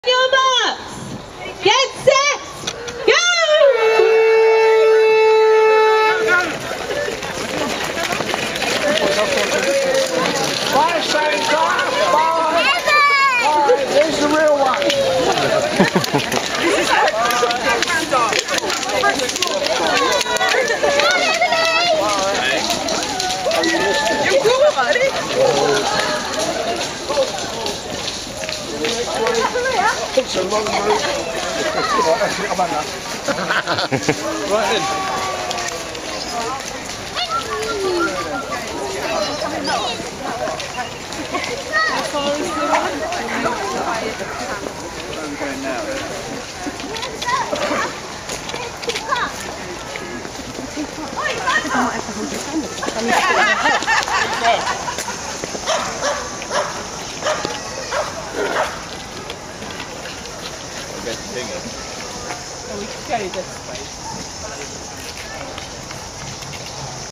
Nonsuch Park Santa Dash 2016 long race start
The start of the race outside Nonsuch Park's mansion house on December 11th 2016, in aid of St. Raphael's Hospice at North Cheam. The sight of 300 Santas running through the park perplexed quite a number of park walkers - both two-legged and four-legged.